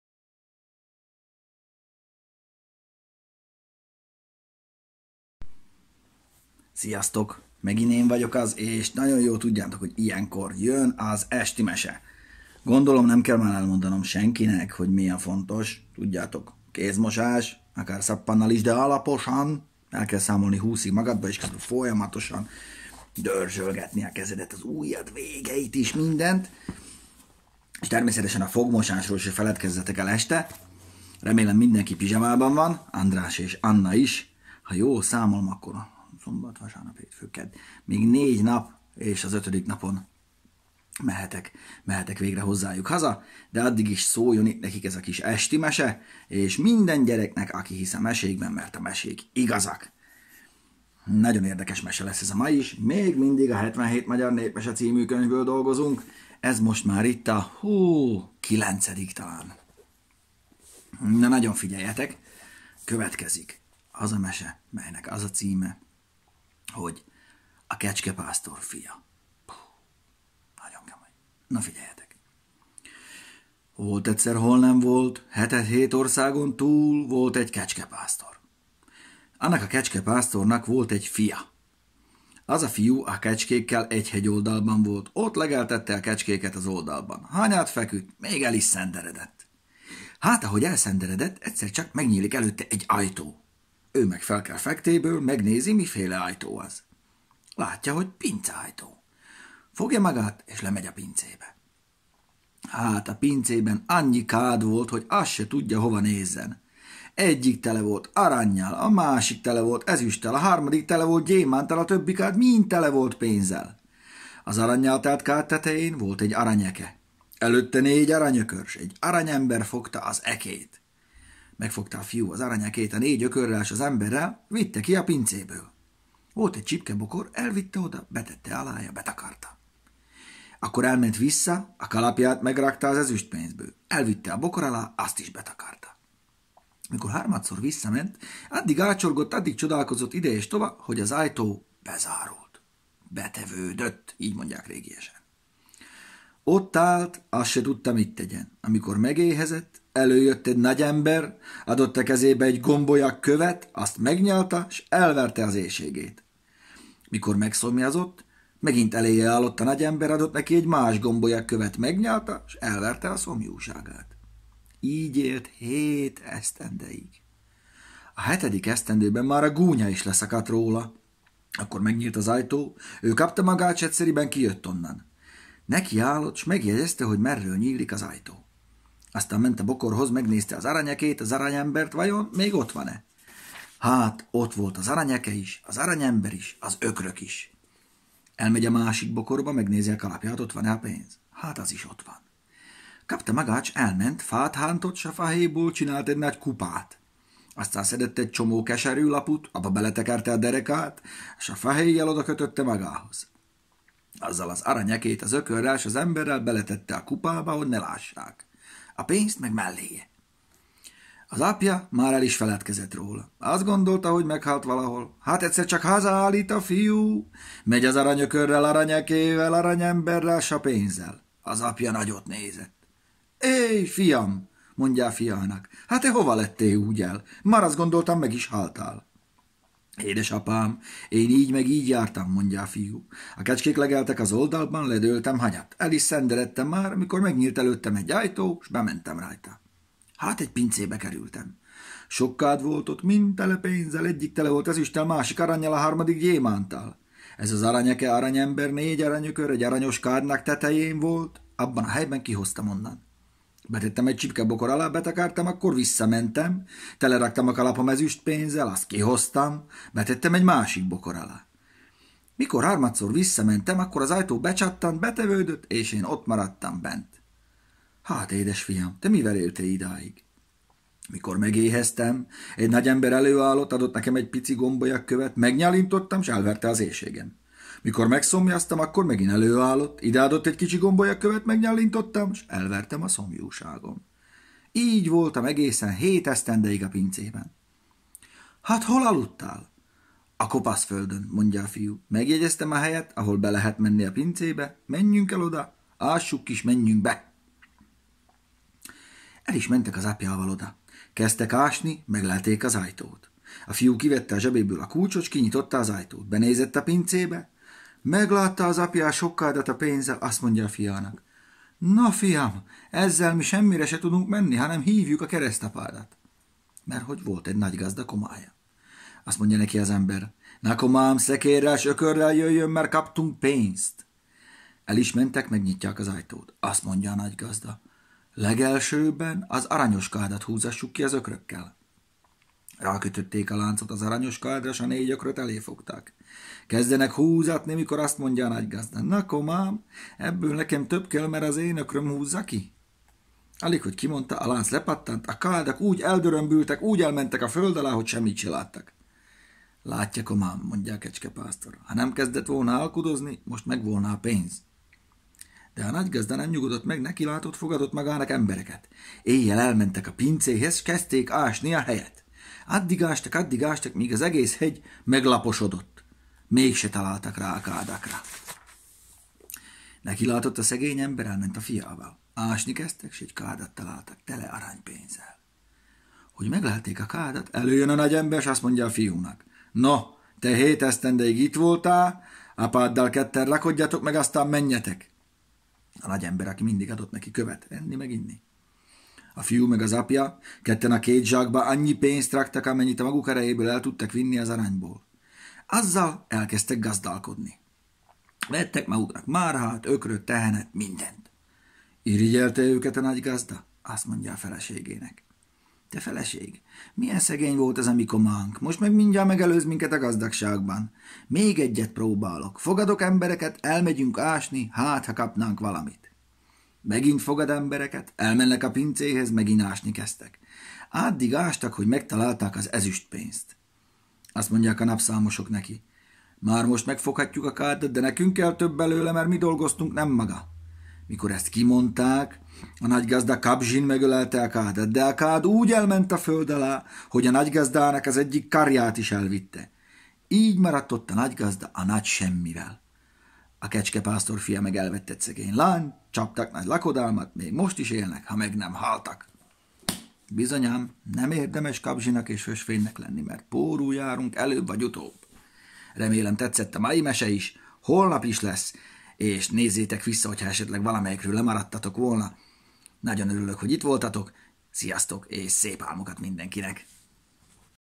Esti mese: A kecskepásztor fia /Illyés Gyula nyomán/
Ismét egy nagyon-nagyon tanulságos történetet mondok el nektek, gyerekeknek, akik figyelemmel kíséritek ezt a kis sorozatot. De legfőképpen Andrásnak és Annának, akik remélem már várnak haza nagyon. Legalább annyira, mint amennyire én szeretnék már otthon lenni.